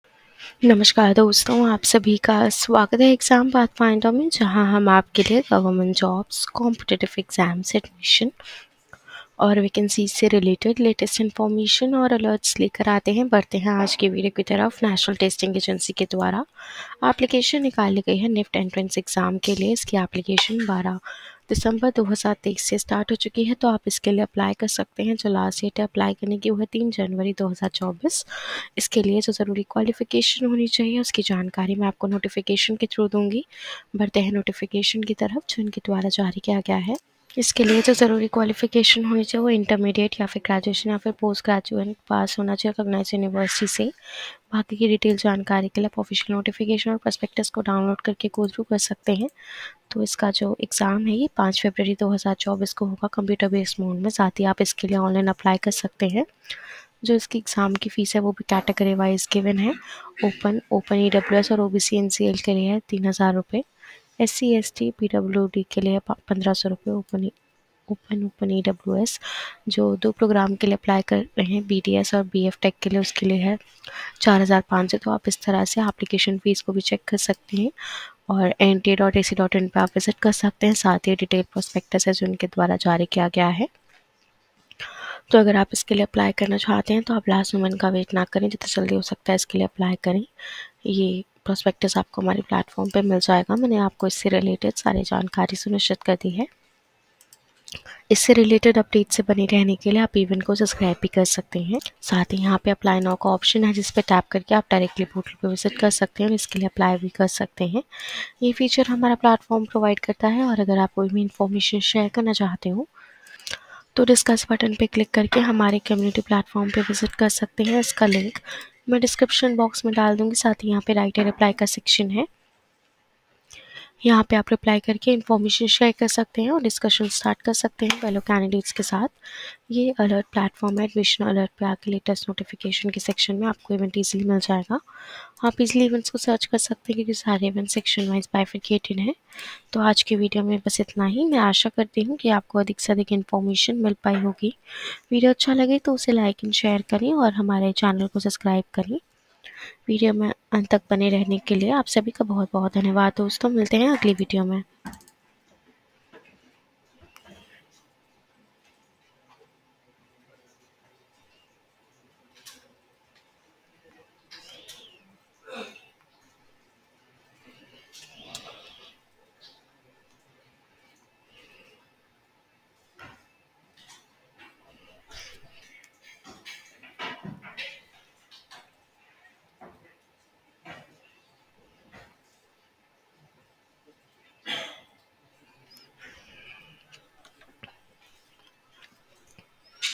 NTA || NIFT Entrance Exam 2024 || APPLY NOW || NOTIFICATION EXPLAINED
ONLINE APPLICATIONS INVITED BY NTA FOR NIFT 2024.
NIFT 2024 KA ONLINE FORM .
NIFT 2024 HOW TO APPLY .